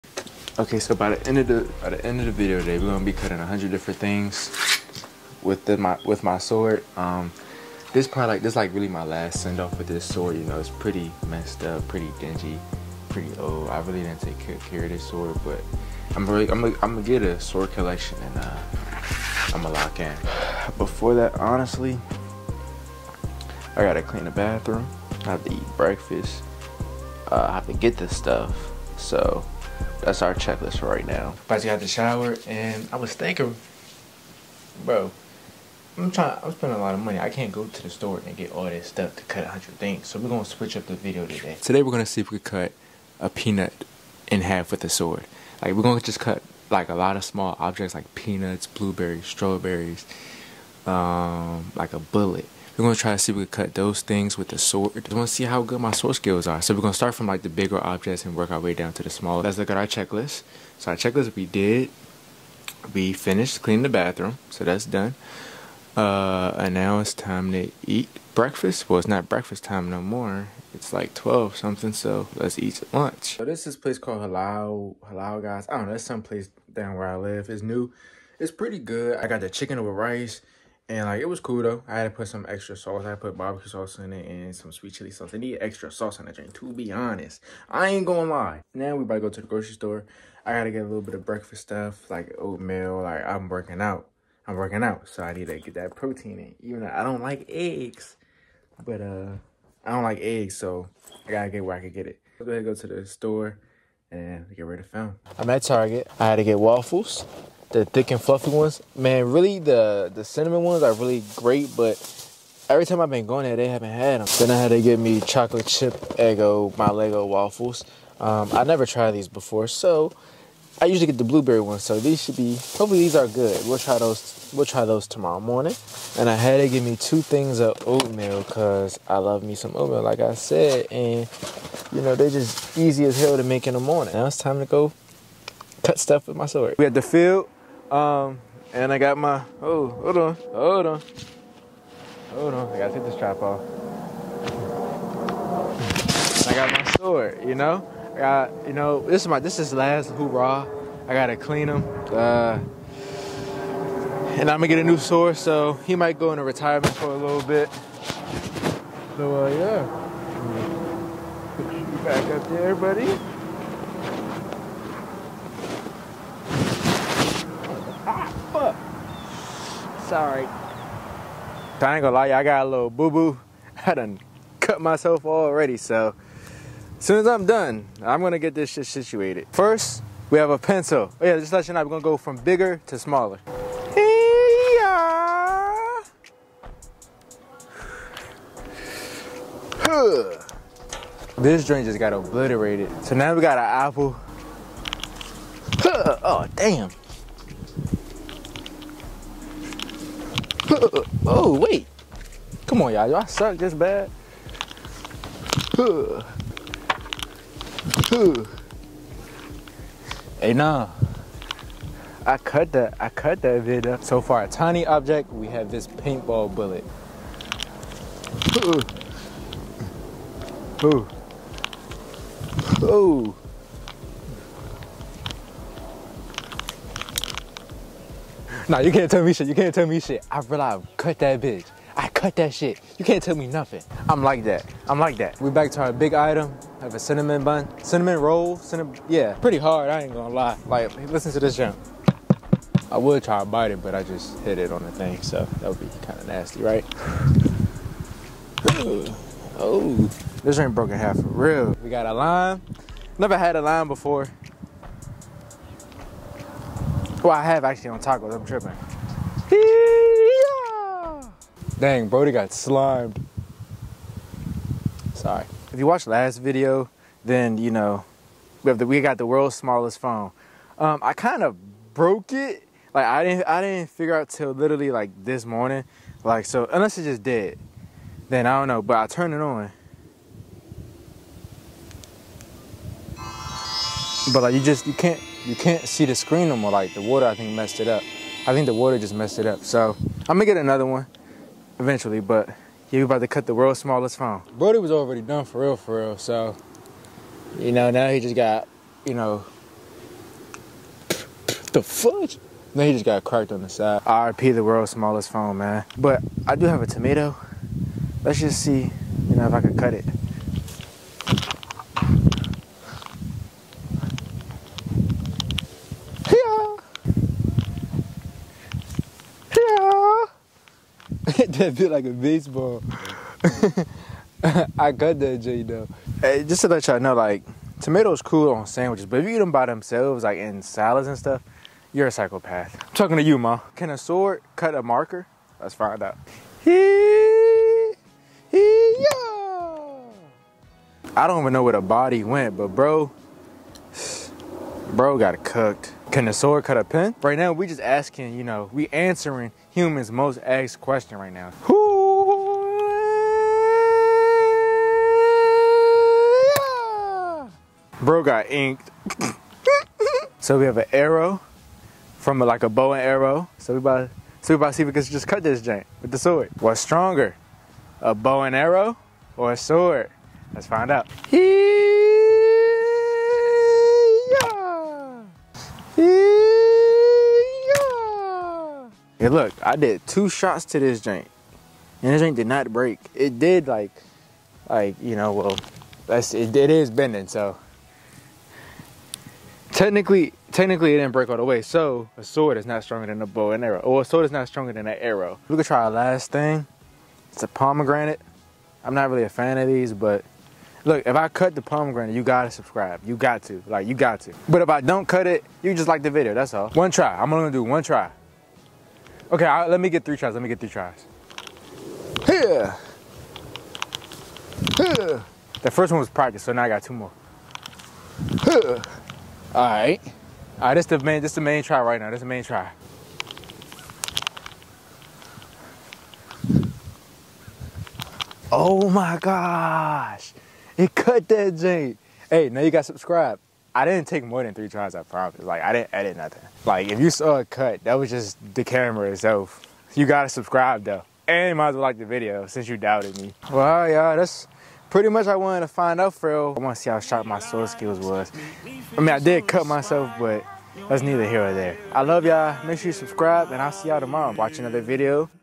Can a Sword Break a Bow n' Arrow?
Can a Sword Break a Bow n' Arrow?. Ju cuts the items with a sword. Ju cuts the items from biggest to smallest.